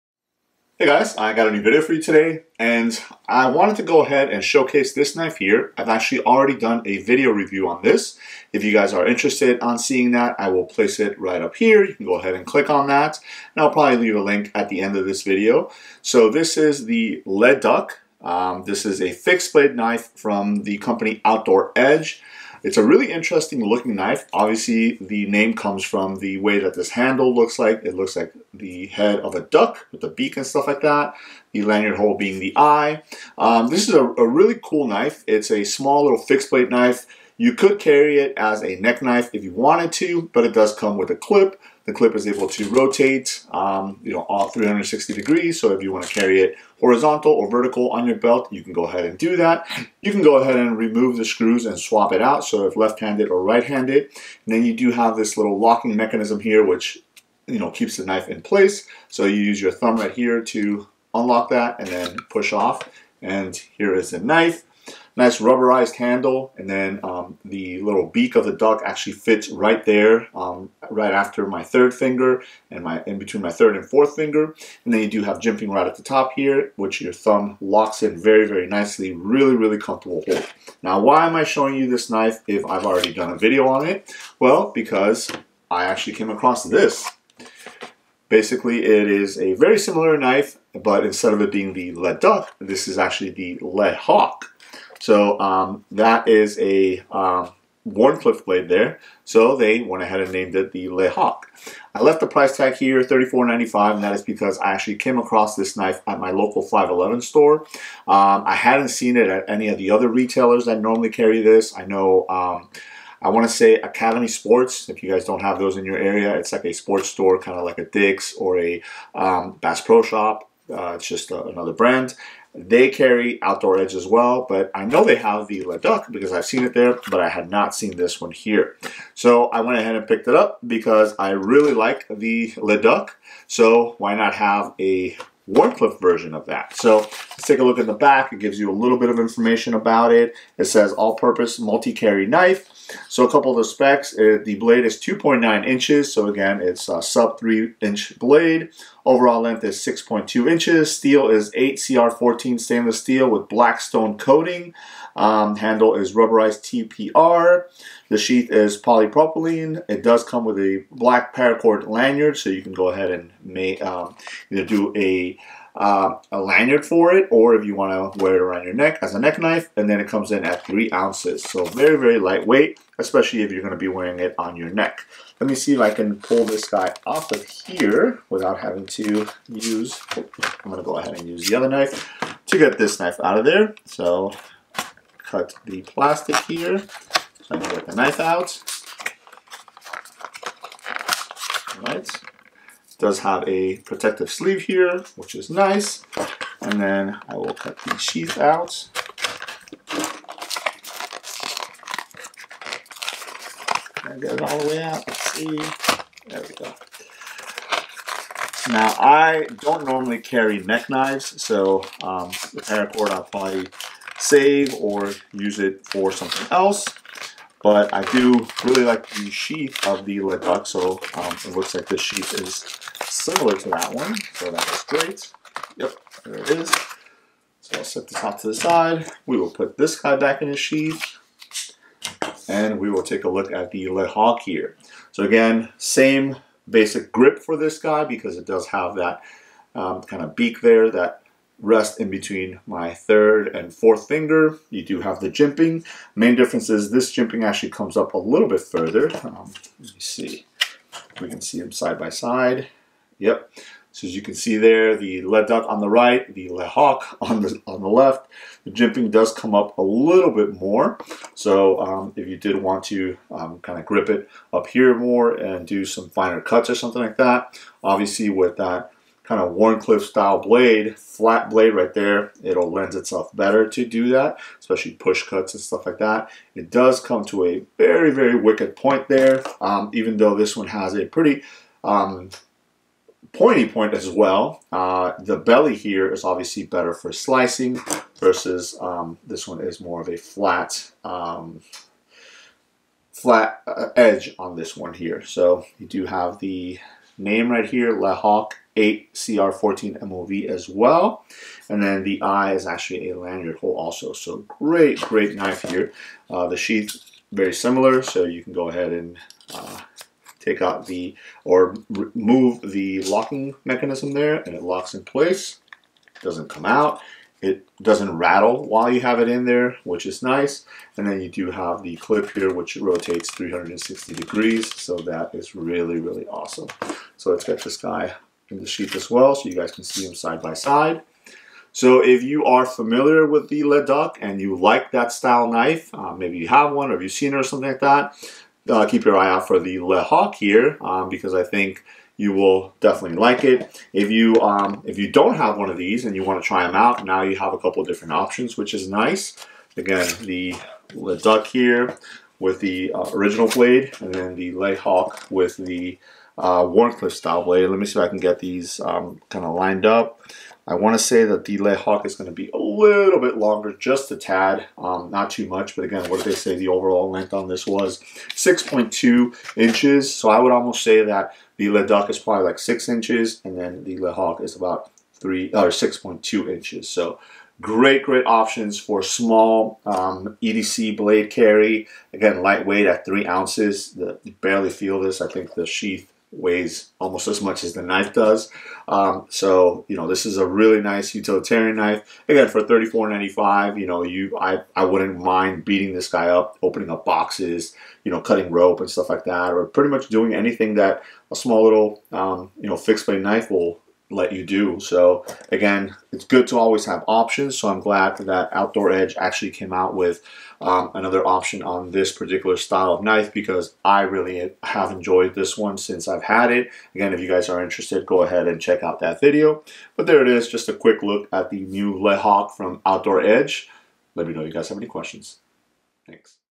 LeHAWK All-Purpose Muti-Carry Knife
A look at the LeHAWK, an great belt or neck carry knife option.